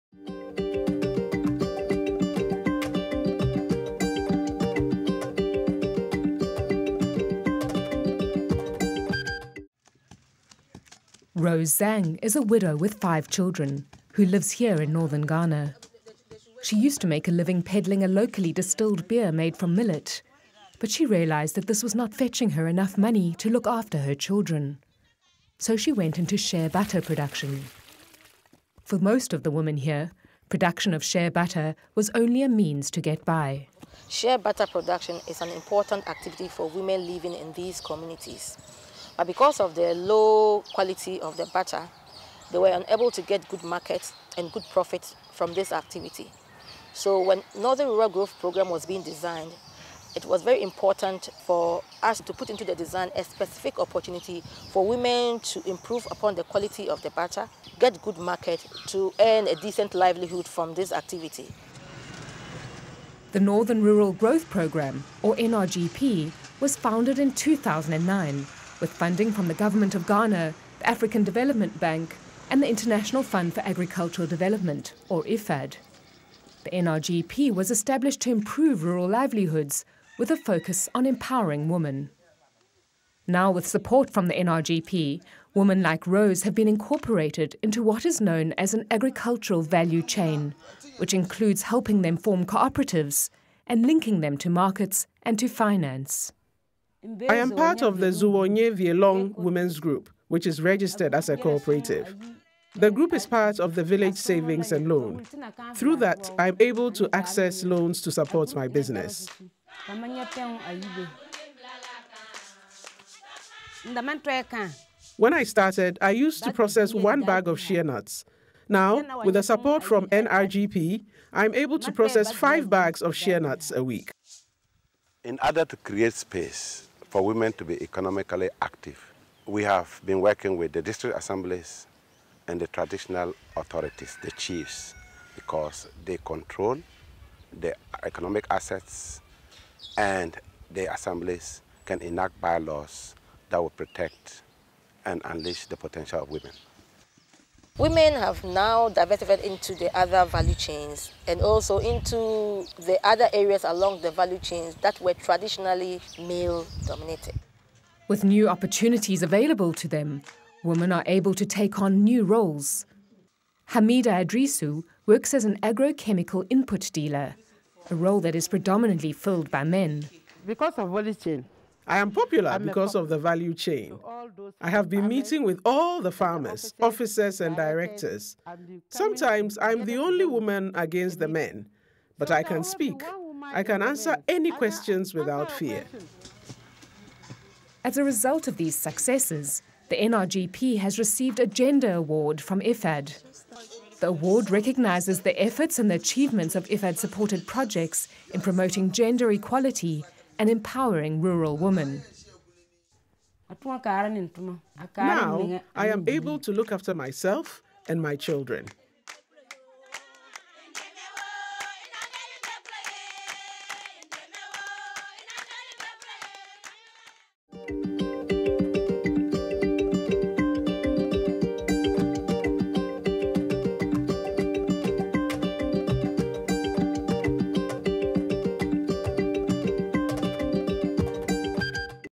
Ghana: Buttering up IFAD (en)
Shea butter production is changing the lives of women in Northern Ghana - such as widow, Rose Zang, who can now support her five children.

Created: 2014

Credits: IFAD